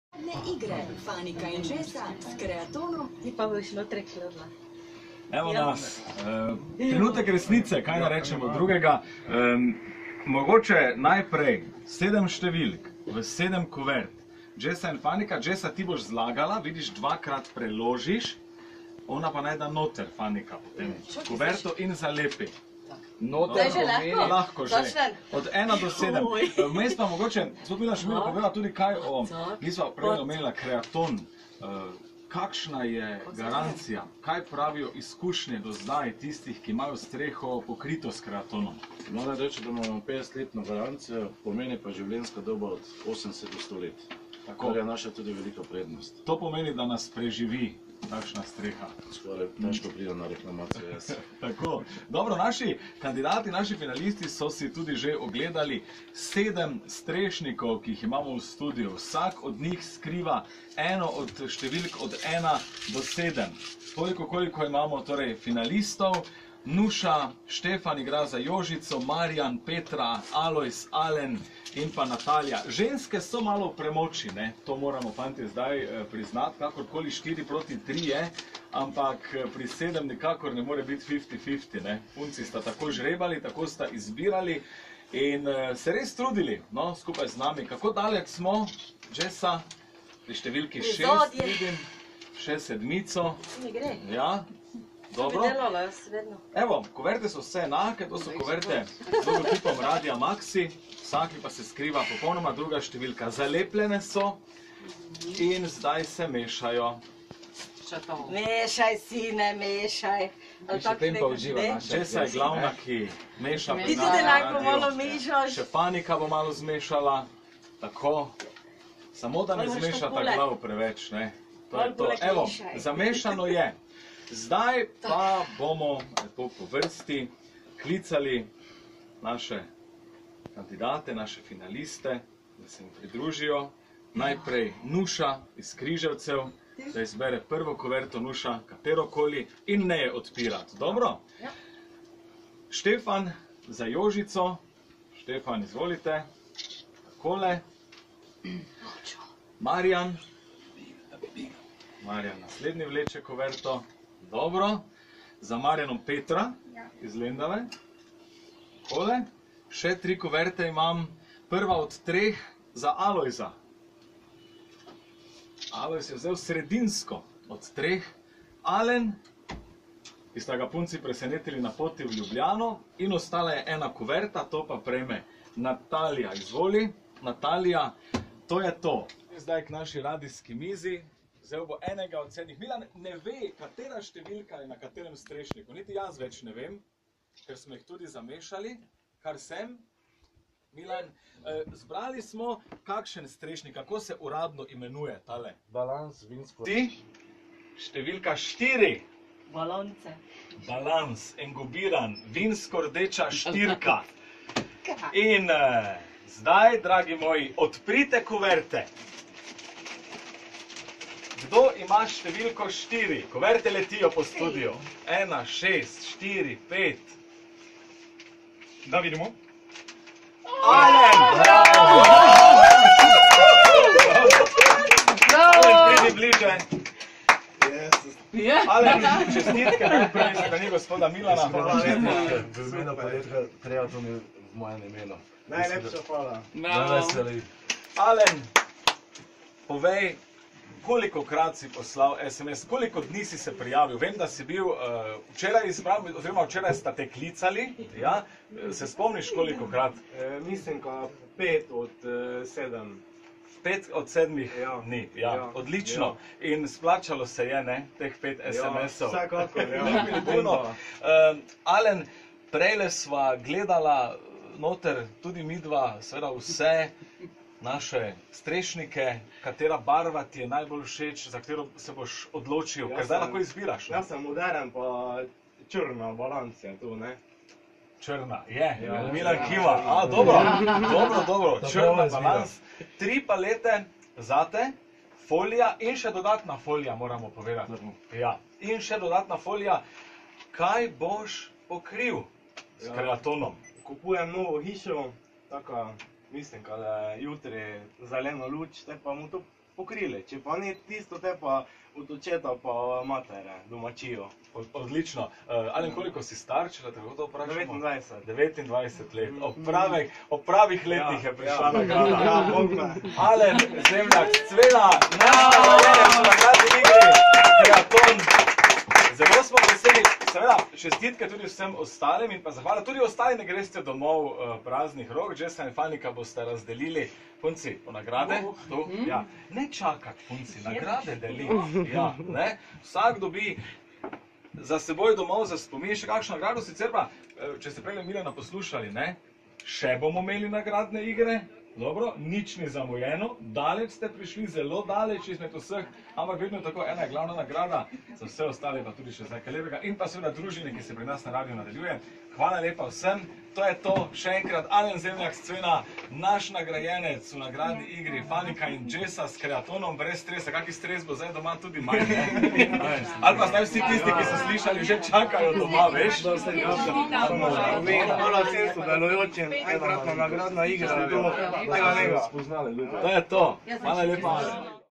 Fanika in Jessa s Creatonom brez stresa
Zaključek nagradne igre - Fanika in Jessa s Creatonom brez stresa - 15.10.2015; radio Maxi